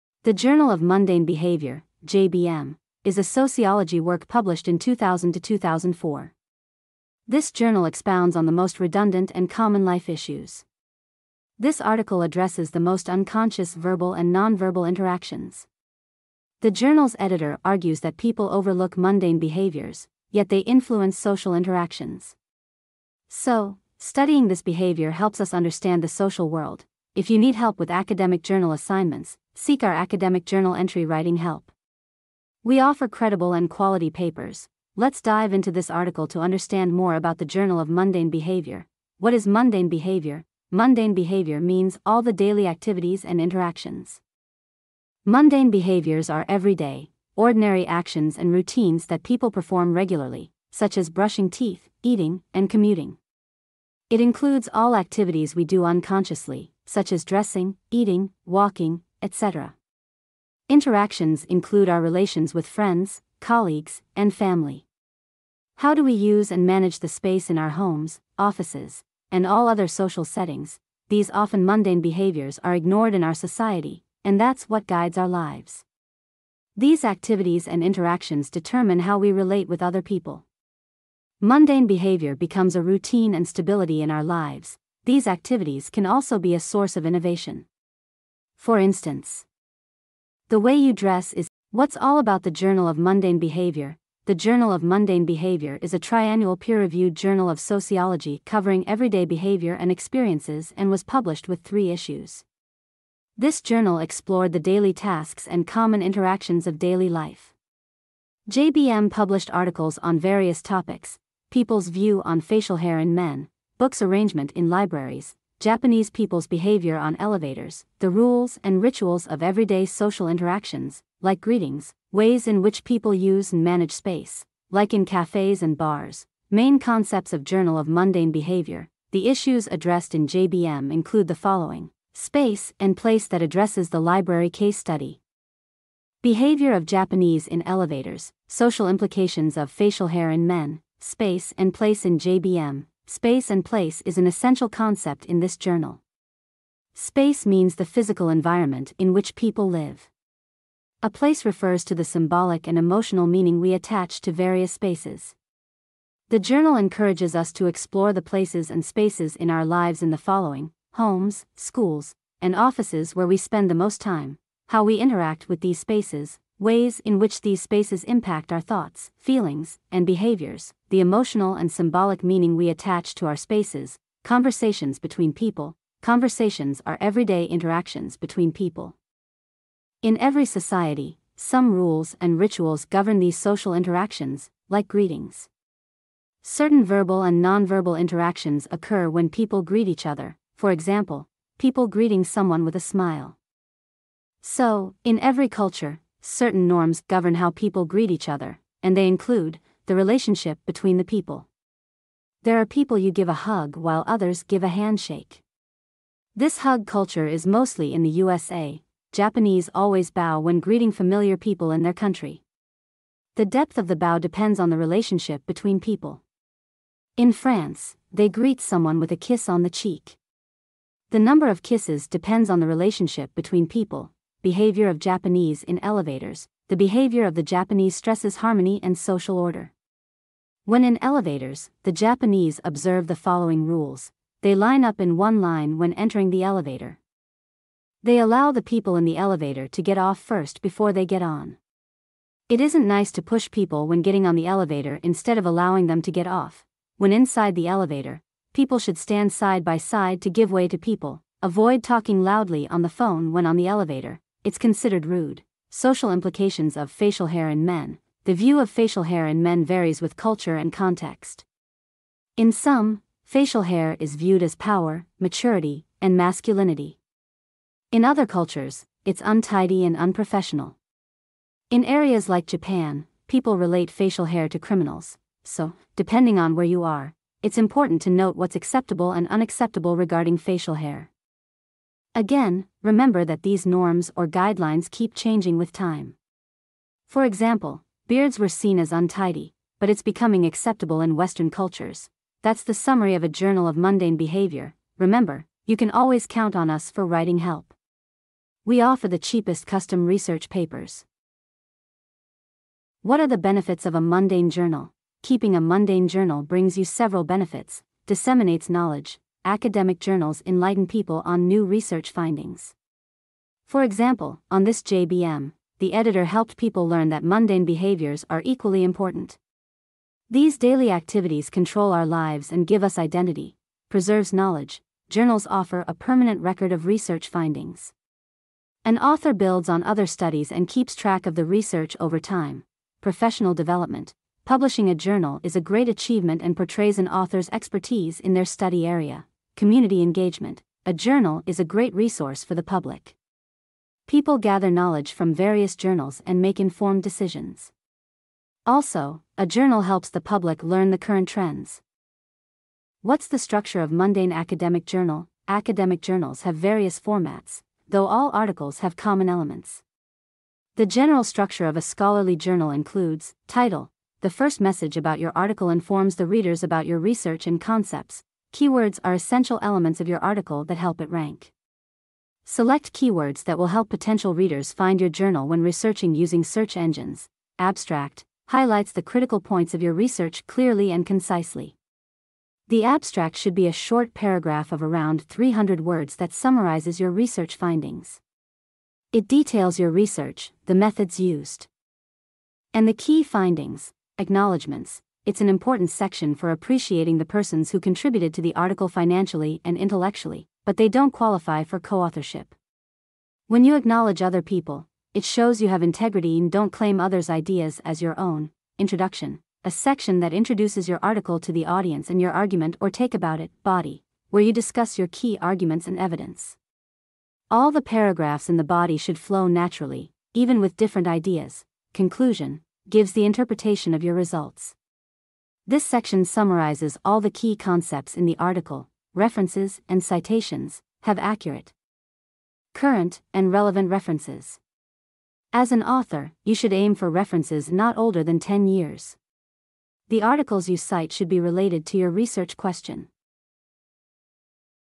Mundane Behavior
Check out the updated version of Mundane Behavior 📚✍🏻

We're delving deep into the world of "Mundane Behavior." You might be wondering, what is mundane behavior, and why should we pay attention to it? Well, you're in for a fascinating journey as we uncover the beauty and significance hidden in the everyday actions that often go unnoticed.

To get direct to the part that you are more interested in follow the time code below🕐:
00:00:00 Intro
00:01:32 What's all about journal of mundane behavior
00:05:02 Benefits
00:05:53 Structure